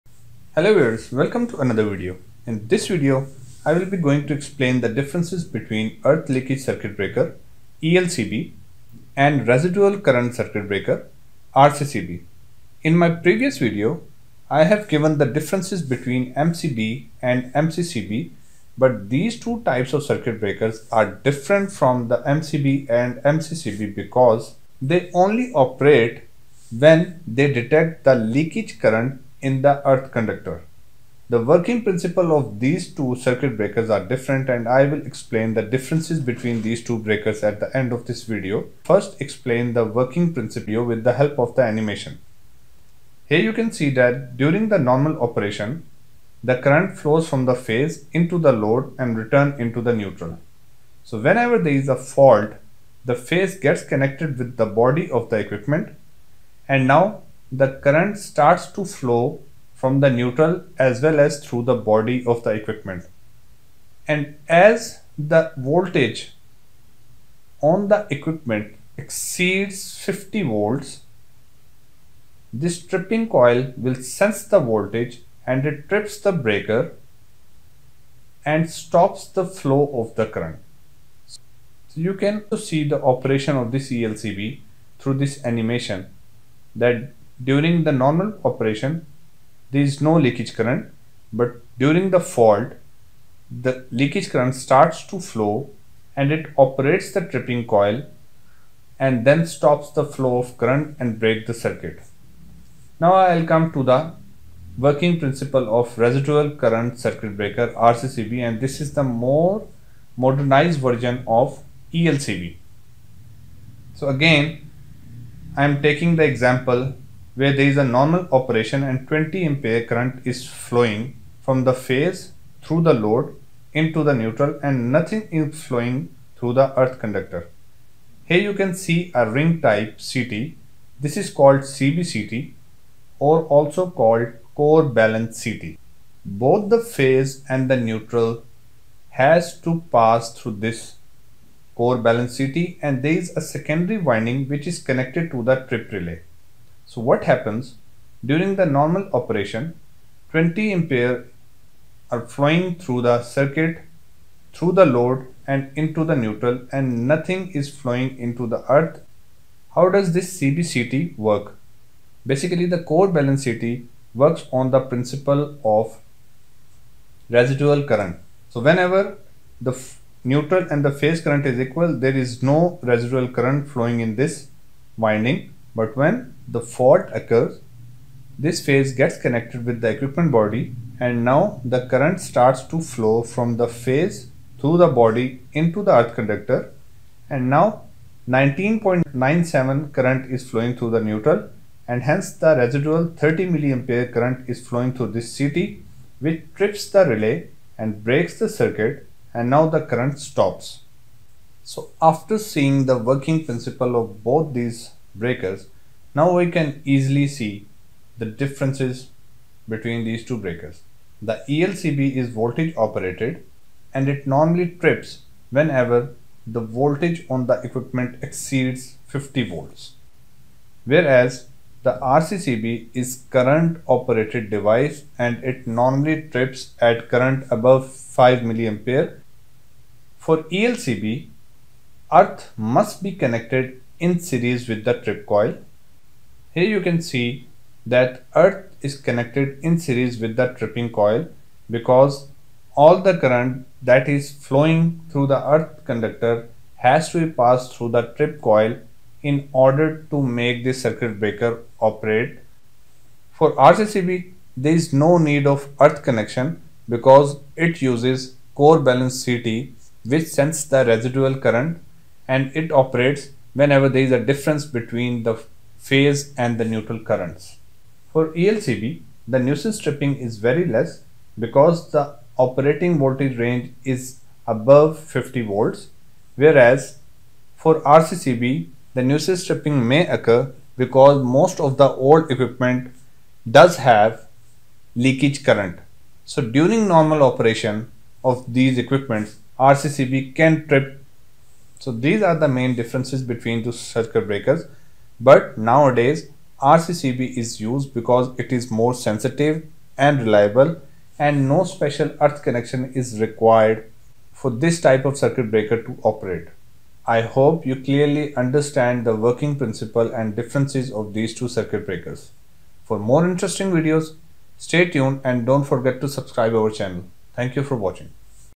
ELCB vs RCCB | Difference between ELCB and RCCB | Animation
RCCB vs ELCB explains the difference between RCCB and ELCB. I have used animation to show the working principle of RCCB and ELCB during normal operation and fault condition.
At the end I have shown the main differences between RCCB and ELCB and why RCCB is now preferred.